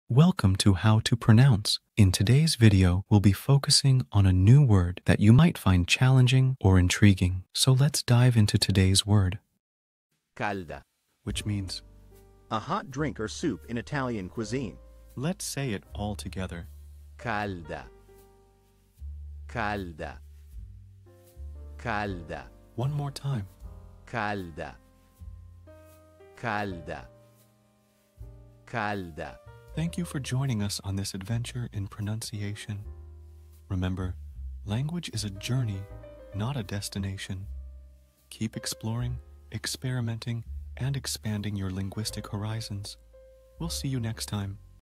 How to pronounce Calda
Master the Pronunciation of 'Calda Which Means hot' - which means : A Hot Drink or Soup in Italian Cuisine. 📝 with @HTPronounce 🎓🗣️ - [Your Guide to Italian]

Learn to pronounce 'Calda' perfectly in Italian with our easy and engaging tutorial by @HTPronounce! 📖📽️ In this comprehensive video, we provide you with native speaker examples 🗣️👂 and phonetic breakdowns 🎵 to ensure you get the pronunciation just right.

How to say Warm in Italian